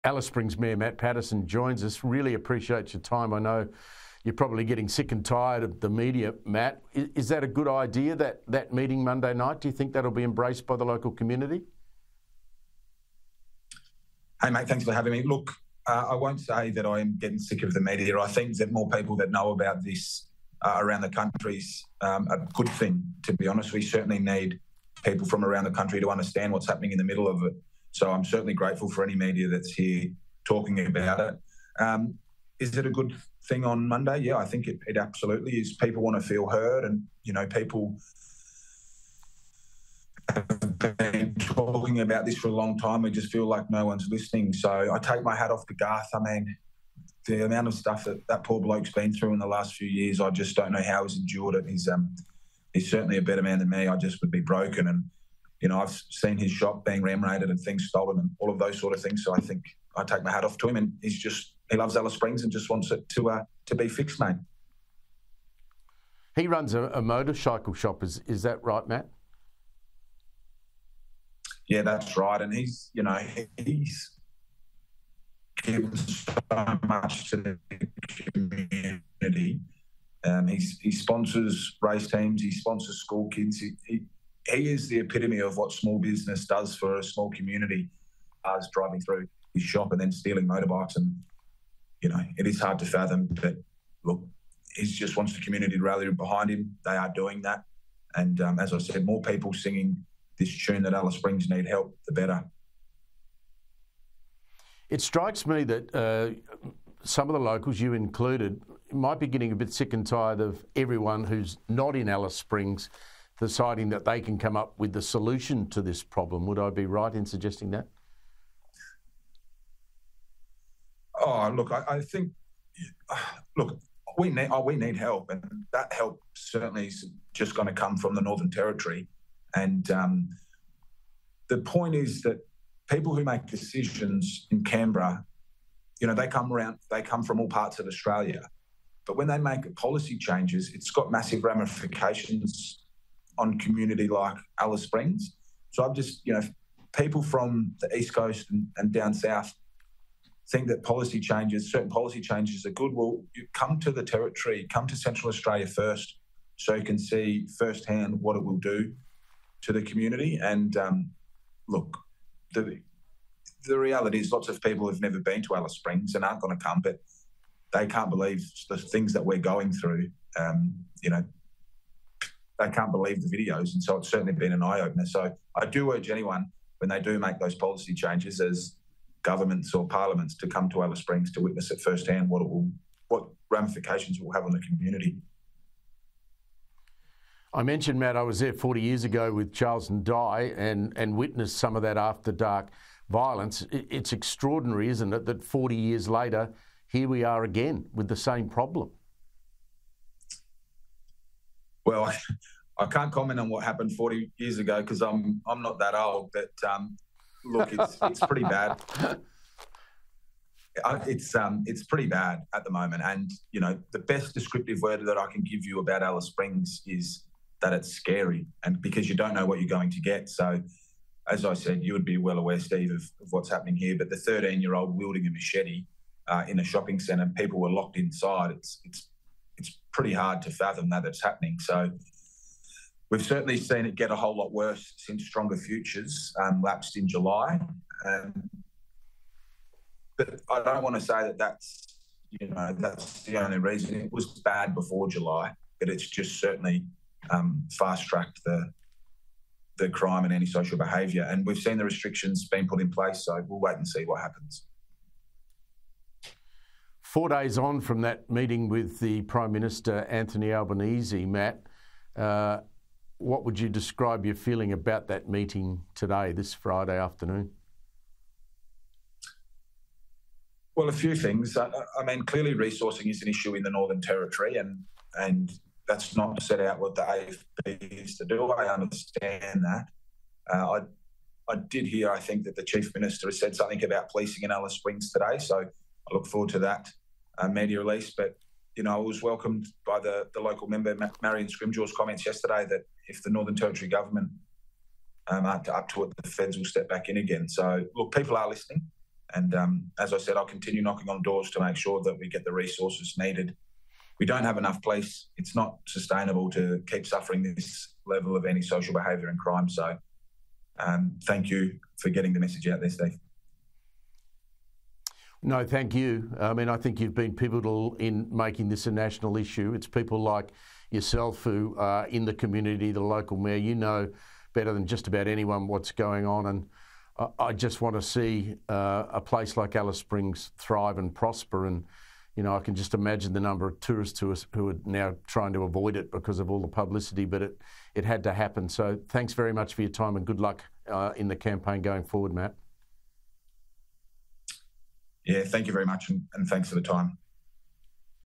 Alice Springs is ‘pretty bad’ at the moment: City Mayor Matt Paterson
Alice Springs is “pretty bad” at the moment, says Alice Springs Mayor Matt Paterson.

“The best descriptive word that I can give you about Alice Springs is that it is scary,” Mr Paterson told commentator Steve Price.

“You don’t know what you are going to get.

“You’ve certainly seen it get a lot worse.”